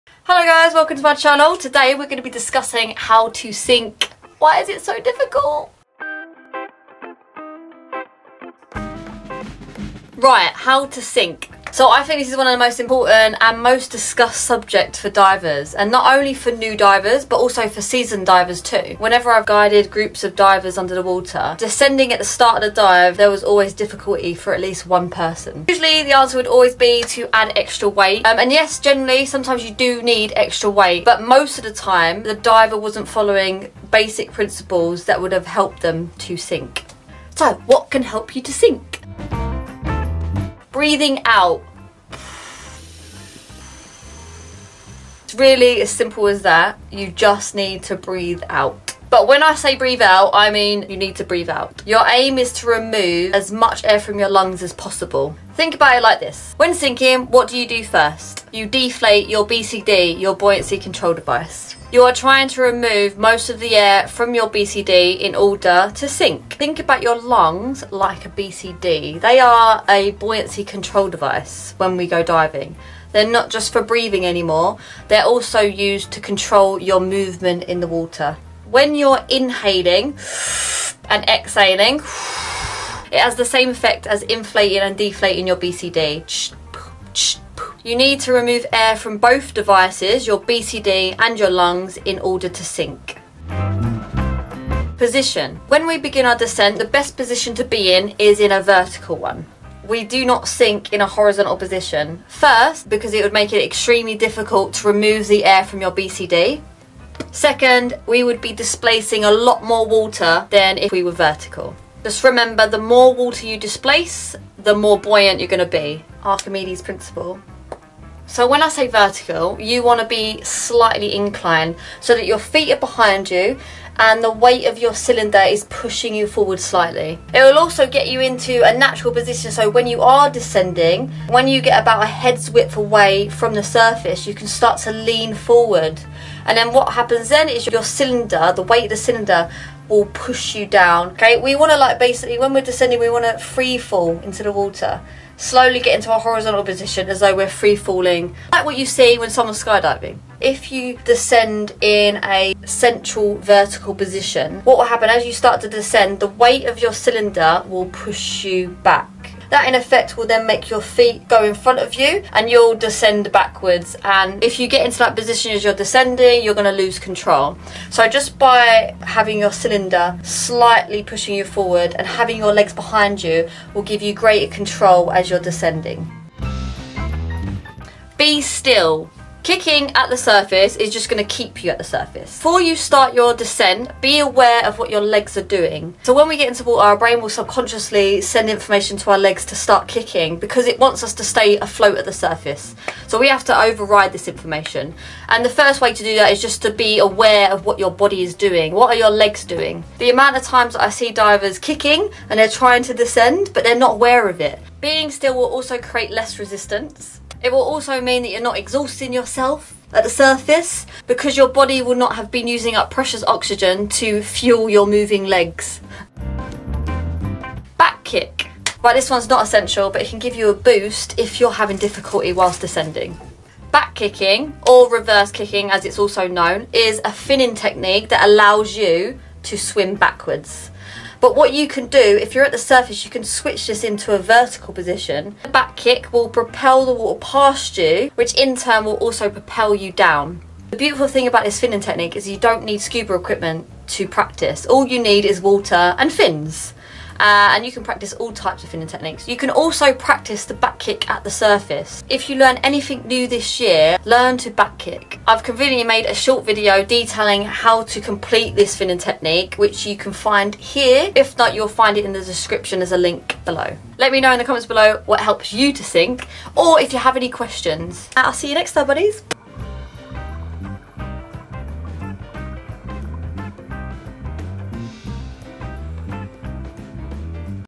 HOW TO descend at the start of your dive | SCUBA SKILLS
How to effectively descend at the start of a dive is always a top topic in scuba diving. Rather than always reaching for more weight, why not try these few basic principles to help you sink!